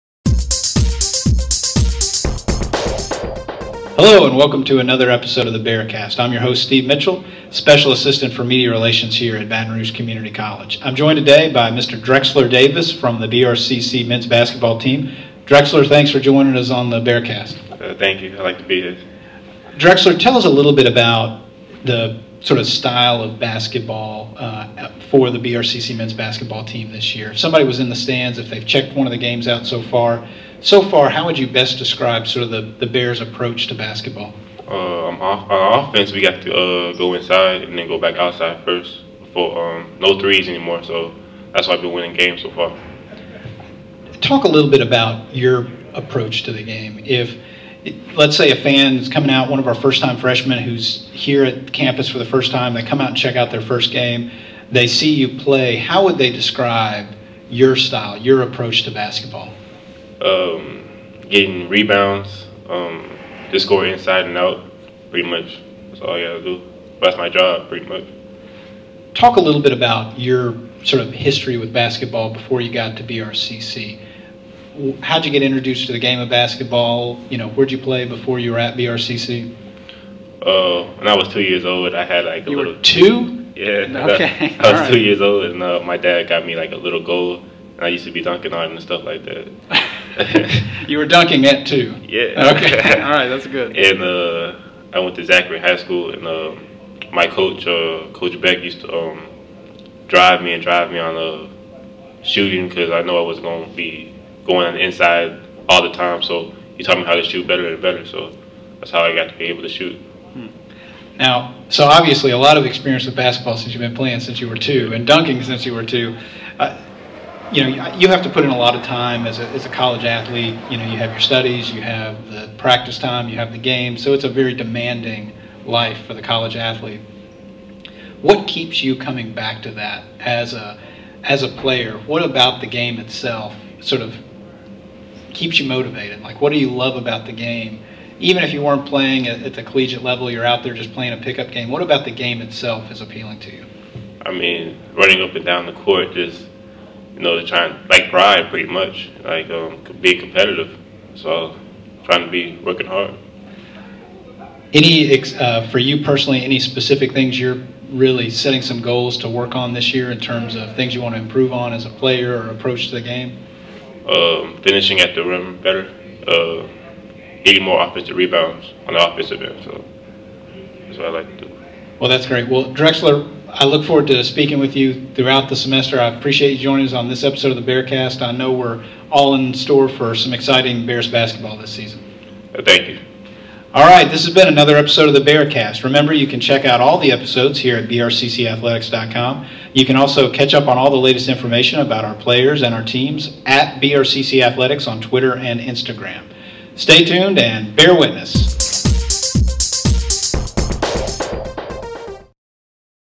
The BearCast for November 17, 2014
We kick off our first BearCast of the 2014-15 season as we catch up with Drexler Davis, #22 for the Men's Basketball Team.  Drexler gives us his take on the 2014-15 Bears and talks about perfecting his dunk as the age of 2!